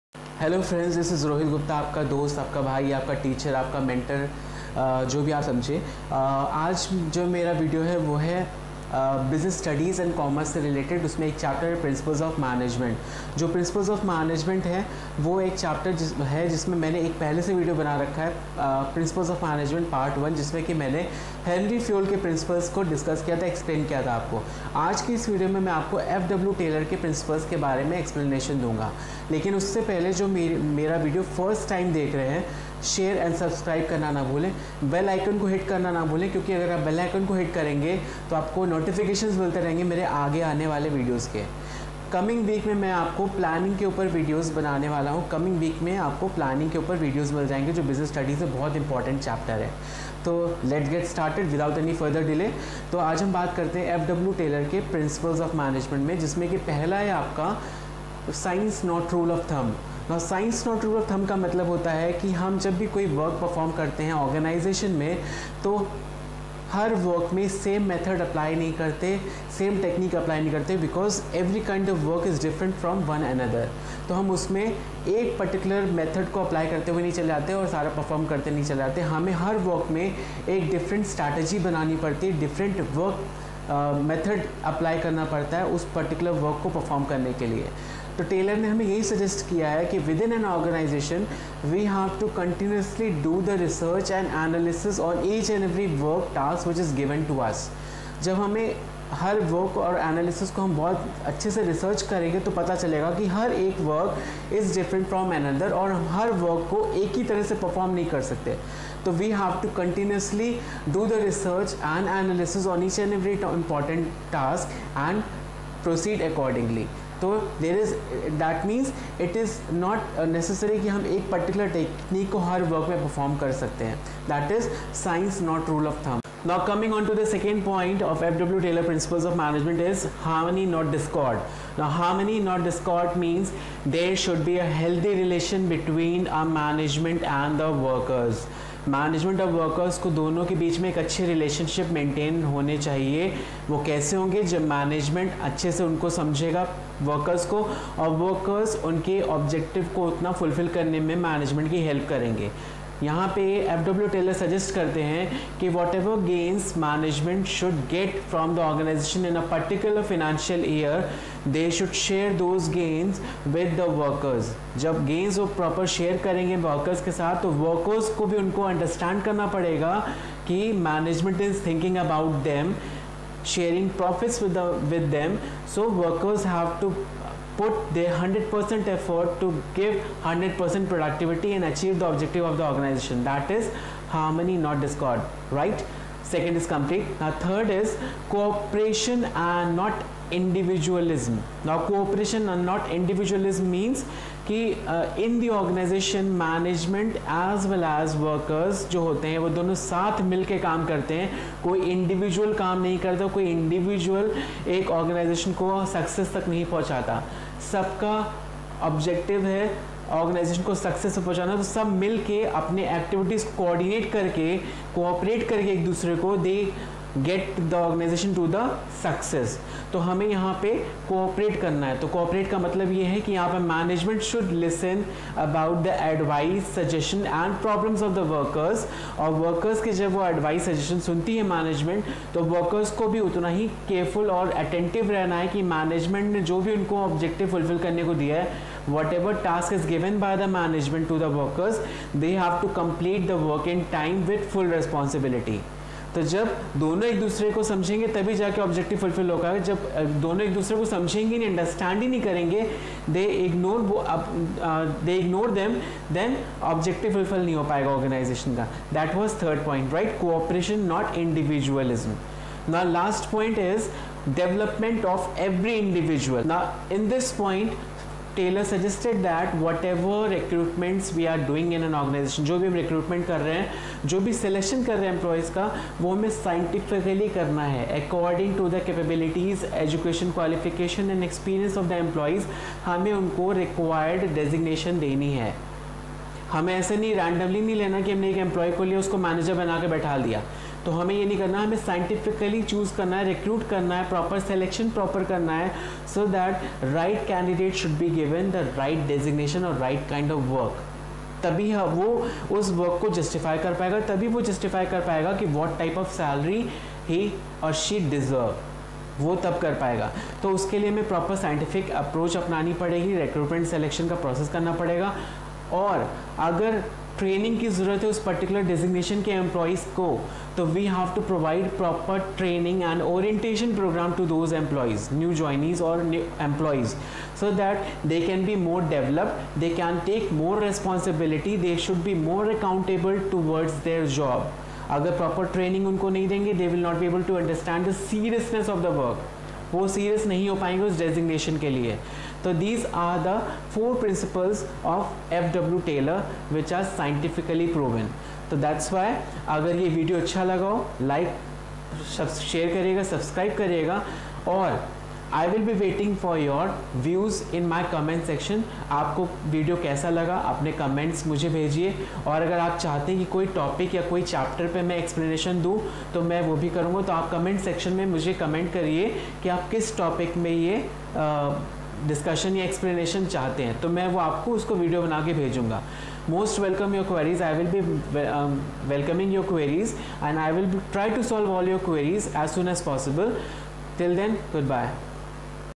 Principles of Management | FW Taylor principles of Management |ROHITG'S COUNSELLING
Hello students in this video I have explained about the Principles of Management by F W Taylor . In my previous video you can se the Fayol's Principles of Management .

However if you have any doubt related to any chapter of Business Studies and Commerce your queries are most welcome.